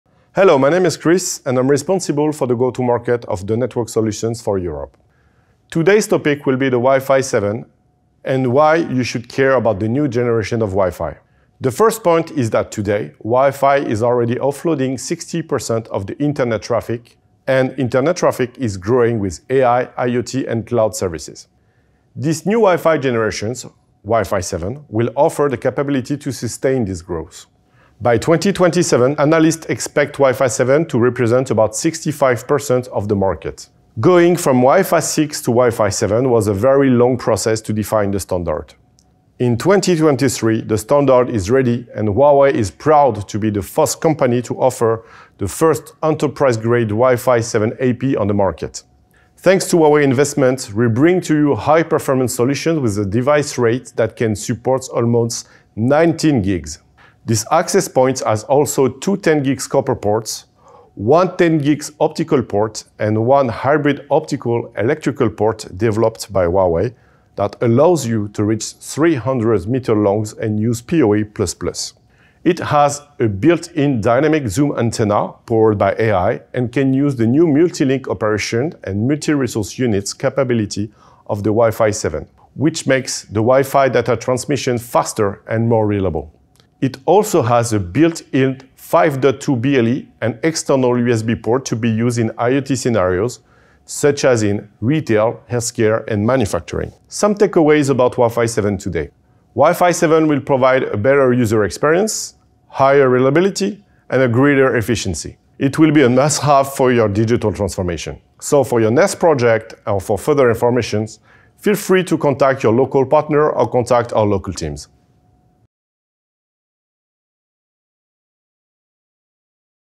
Huawei AirEngine Wi-Fi 7
Introducing the future of Wi-Fi with Huawei's next-generation Access Points!
Experience seamless connectivity and blazing fast speeds up to 18.67 Gbps, perfect for immersive XR applications like remote collaboration, telemedicine & interactive teaching. Enjoy exclusive discounts today.